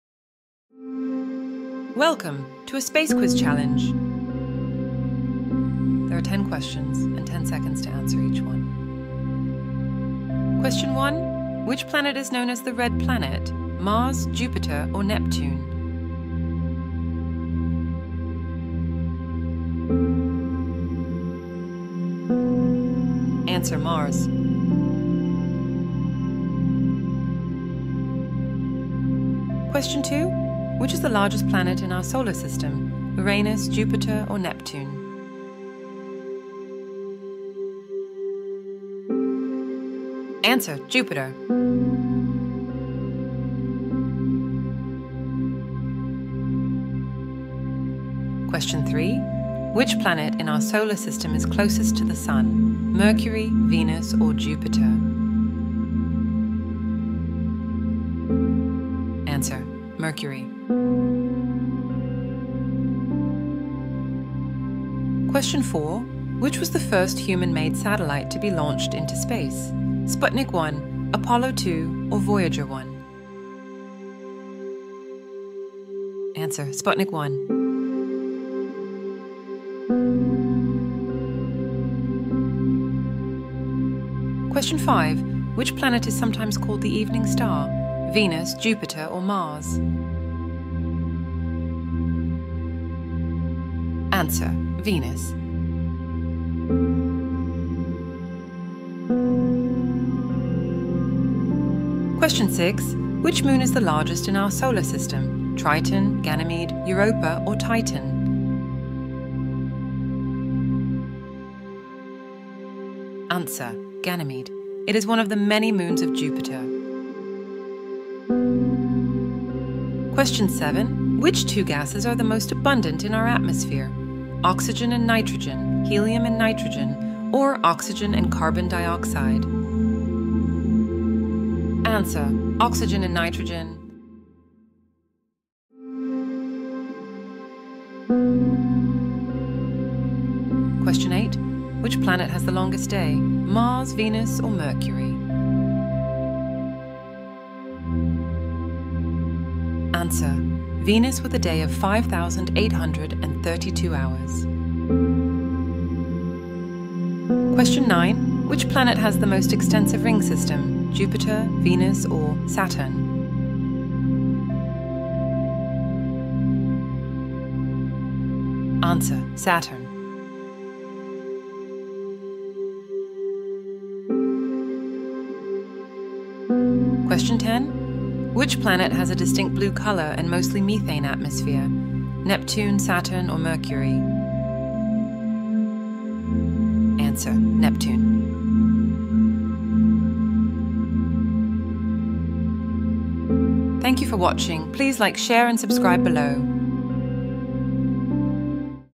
"Galactic Genius: A Celestial Challenge on Planets and Moons!"
Embark on an interstellar journey with "Galactic Genius," a quiz that explores the mysteries of planets and their celestial companions. Test your cosmic knowledge as you orbit through questions about our solar system's diverse worlds and their captivating satellites. From the fiery atmospheres of gas giants to the icy landscapes of moons, this celestial challenge will launch you into a universe of fascinating facts. Are you ready to become a true Galactic Genius? Music created in co-operation with Tracksy.AI.